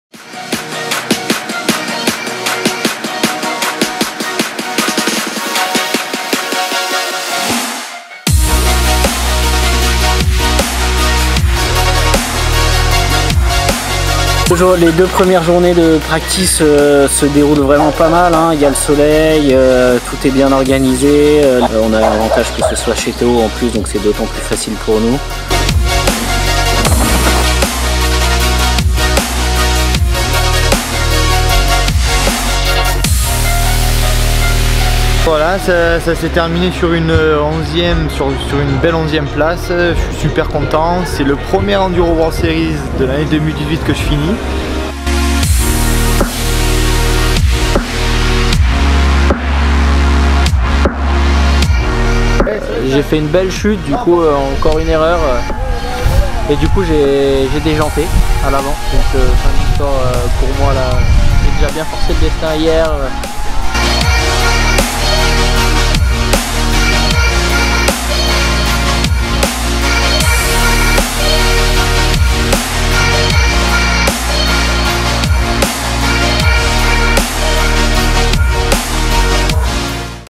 TEAM SUNN ENDURO - Olargues
Retrouvez le TEAM SUNN ENDURO Au Orlagues lors des Enduro World Series 2018. Retour en images sur ce périple.